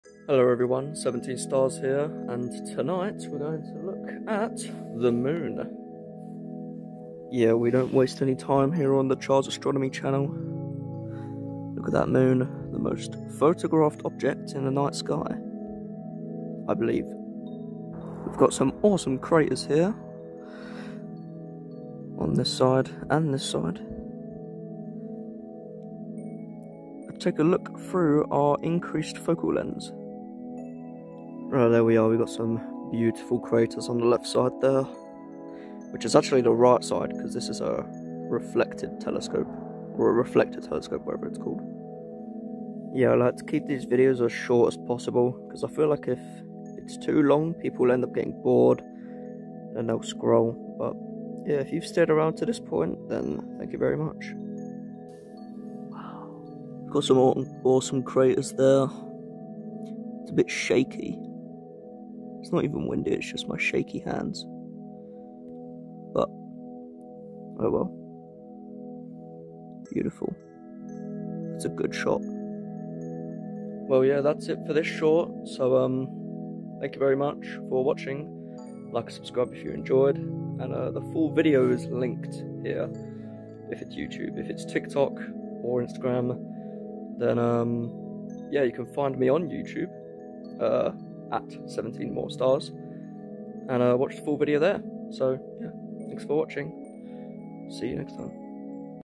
Last Quarter Moon
In this video: I take a look at the moon with my dobsonian telescope. Its a last quarter tonight so hopefully we will see nice colors and craters. I also decided to talk in this video considering I dont normally talk in my astronomy videos.
I make content similar to this type of stuff so help an ameteur astronomer out and sub for more :)

This video was Created on the 13th September But was uploaded on the 20th September